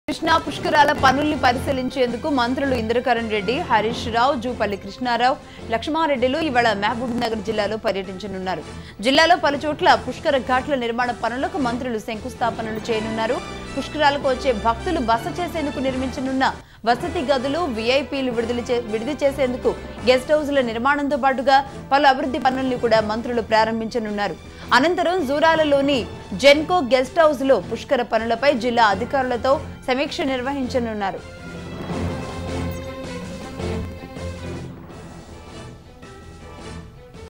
Telangana Ministers To Tour In Mahabubnagar | Krishna Puskara Ghat Works | HMTV
Telangana Ministers Harish Rao, Indrakaran Reddy, Jupally Krishna Rao, Laxma Reddy will tour in Mahabubnagar District today, to review  Pushkar Ghats for Krishna pushkaralu and will inspect the construction works and arrangements at Pushkar ghats for the devotees.